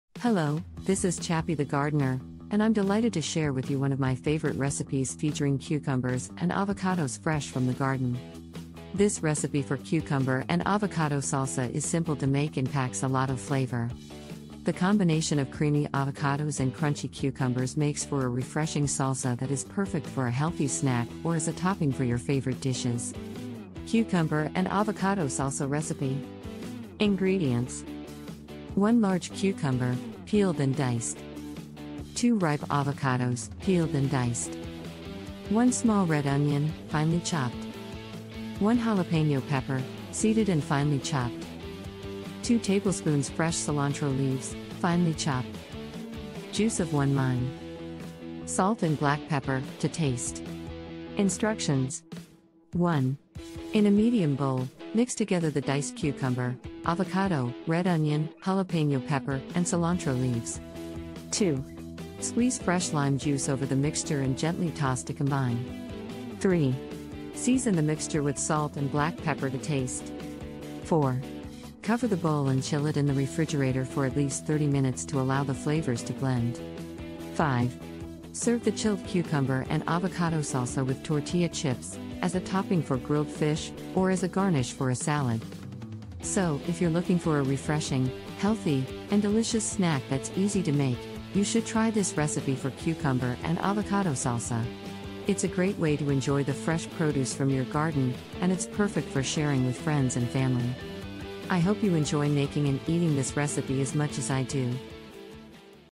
Cucumber and Avocado Salsa
Hello, this is Chappy the gardener, and I'm delighted to share with you one of my favorite recipes featuring cucumbers and avocados fresh from the garden!

This recipe for Cucumber and Avocado Salsa is simple to make and packs a lot of flavor.

The combination of creamy avocados and crunchy cucumbers makes for a refreshing salsa that is perfect for a healthy snack or as a topping for your favorite dishes

Click To Grow